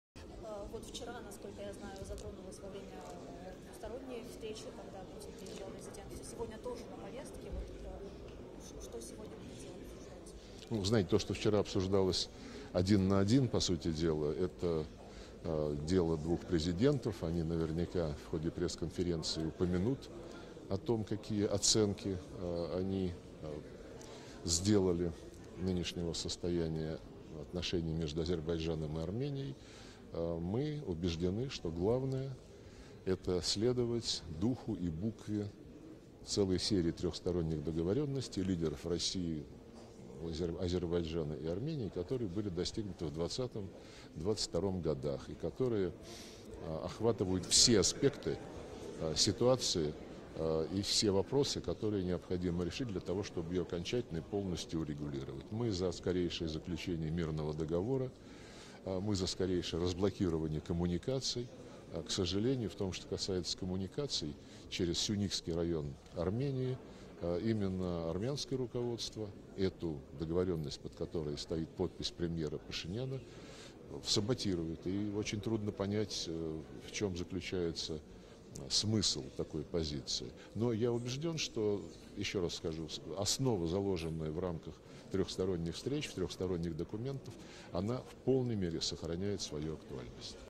ՀՀ ղեկավարությունն է սաբոտաժի ենթարկում համաձայնագիրը Սյունիքով հաղորդակցությունների հարցով. Լավրով
Video credite: @midrftube